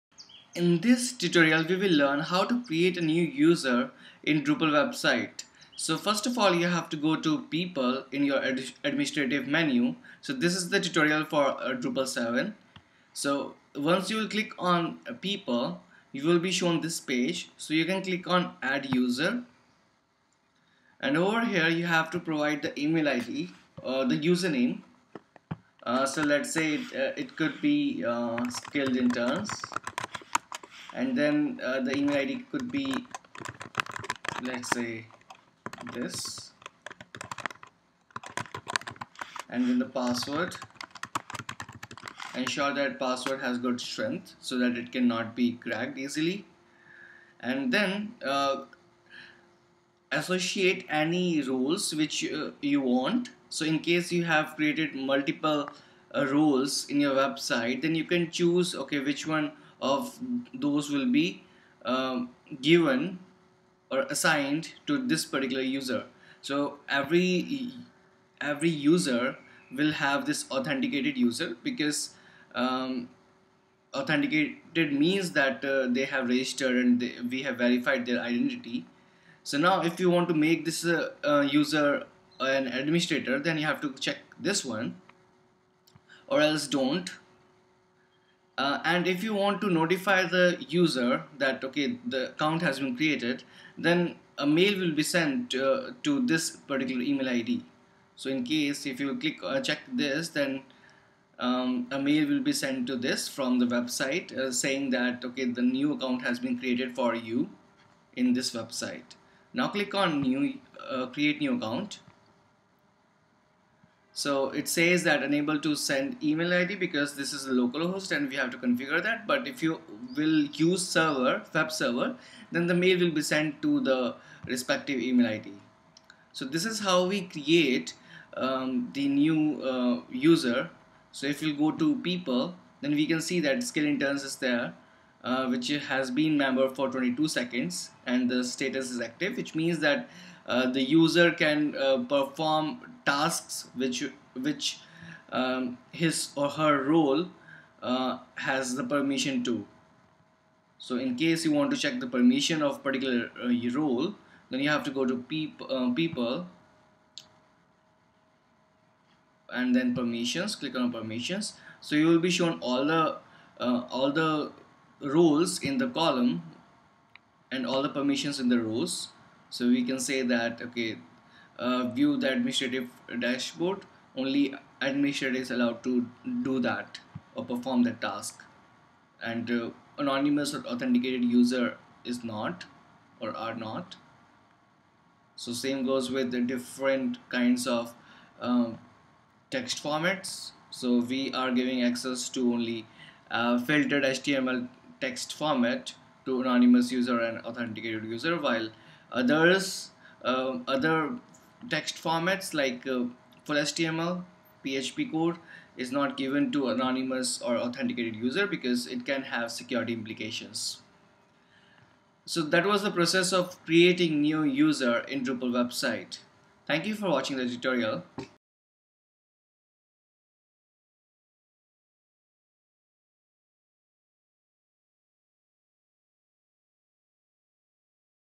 How to create a new user in Drupal 7 ?
Learn User management in Drupal 7 by watching this Drupal Tutorial.